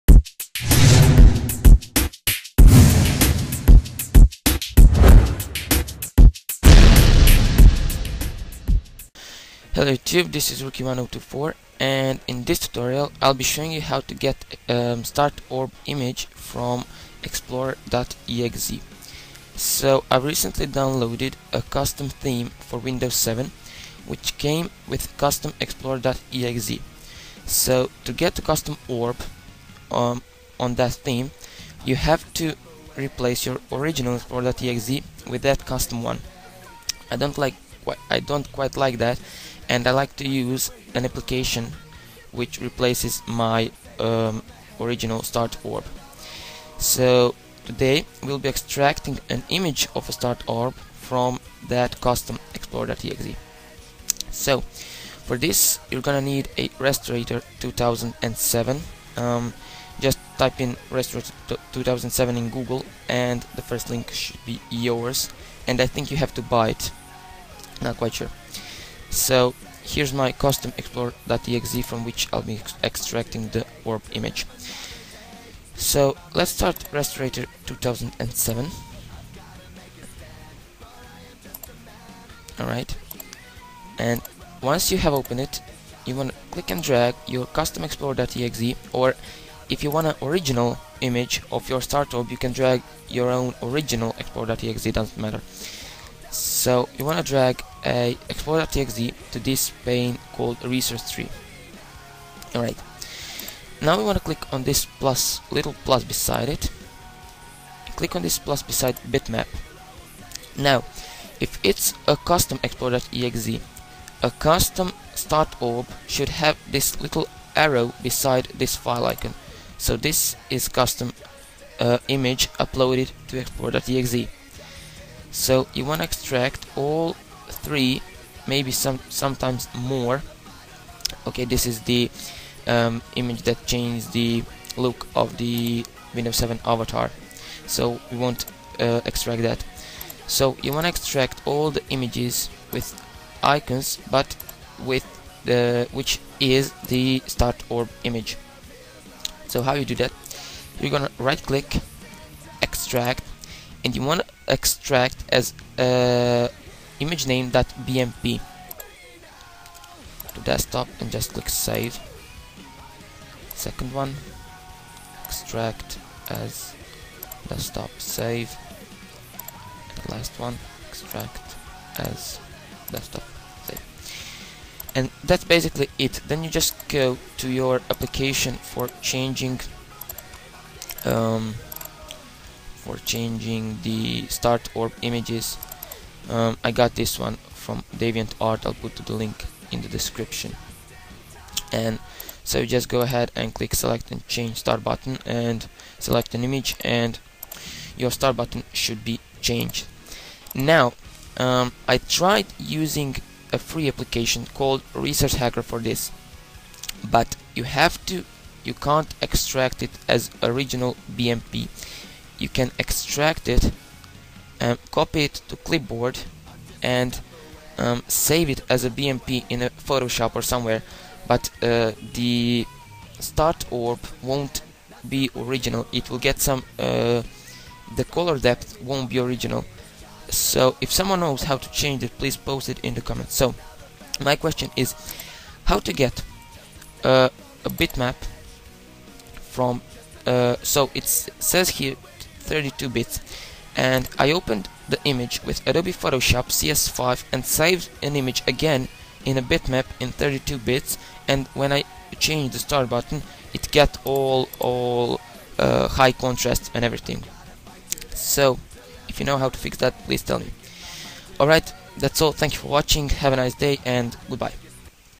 HOW TO...Extract Start Orb Images from explorer.exe and Replace Start Orb
This video will be helpful to all the people that don't want to replace original explorer.exe with custom one, but still want to change their start orb in Windows 7.

WARNING!!! I'M NOT RESPONSIBLE FOR ANY MALICIOUS WORKING OR ANY DAMAGE CAUSED!!! YOU'RE DOING THIS AT YOUR OWN RISK!!!